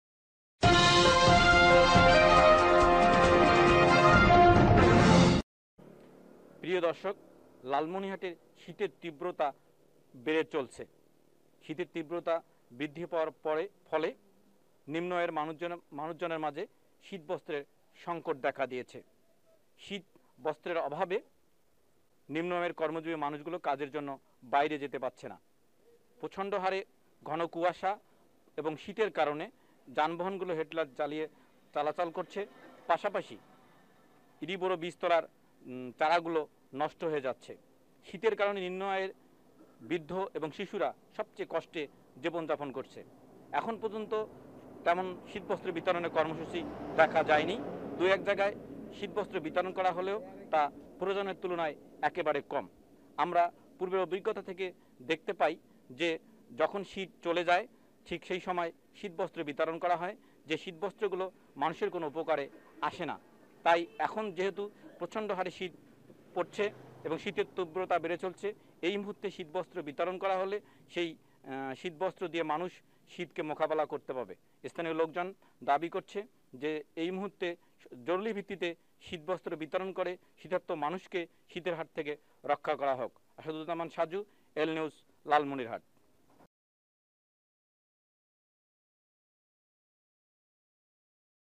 বাড়ছে শীতের তীব্রতা, নেই শীতবস্ত্র বিতরণের উদ্যোগ | L News Online
বাড়ছে শীতের তীব্রতা, নেই শীতবস্ত্র বিতরণের উদ্যোগ
L_News_Online Winter_News